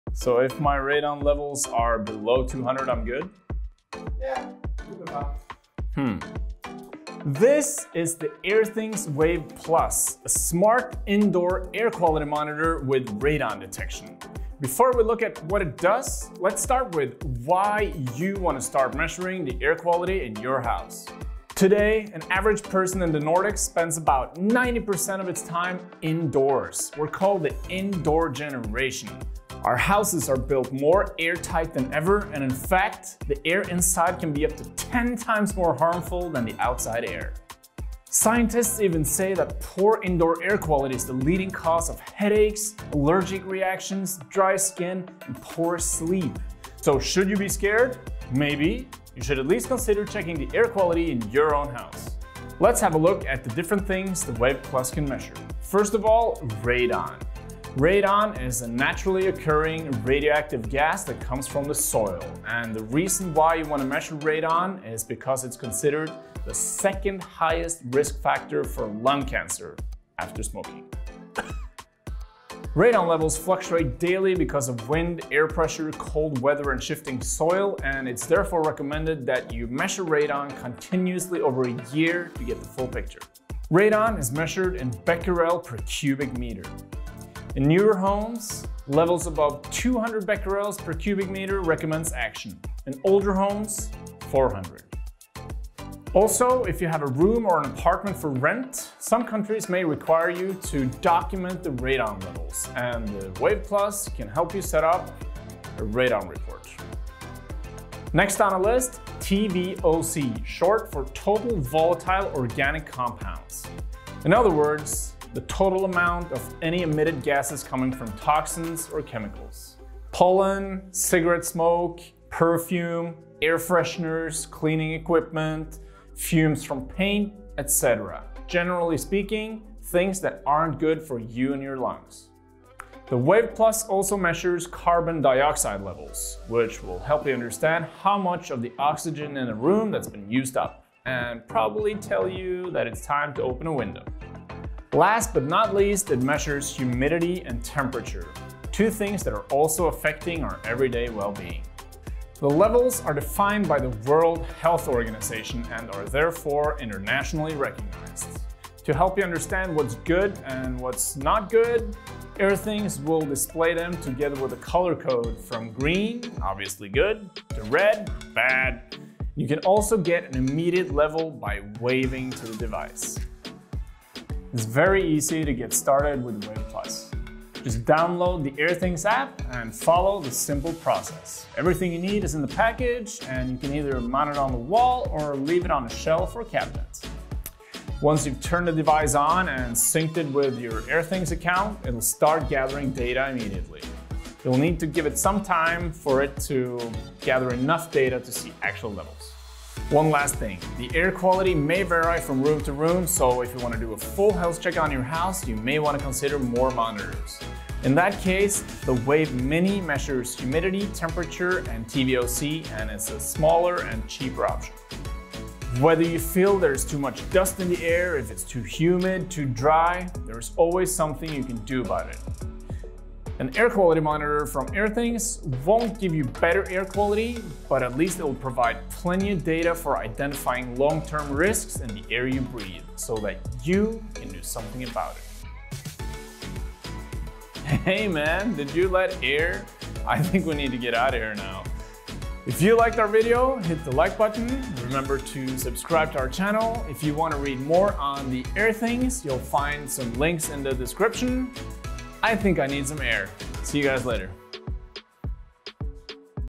How to measure air quality with AirThings
Forskare säger att dåligt inomhusklimat bidrar till huvudvärk, allergi, torr hud och dålig sömn. Med AirThings Wave Plus kan du mäta radonnivå och övervaka luften över en längre period så att du kan lära dig allt om den, och så att du kan åtgärda eventuella problem med den.

Radon är en naturligt förekommande gas som kommer från marken. Varför man vill känna till radonnivåerna är för att det räknas som den näst störst bidragande orsaken, efter rökning, till lungcancer.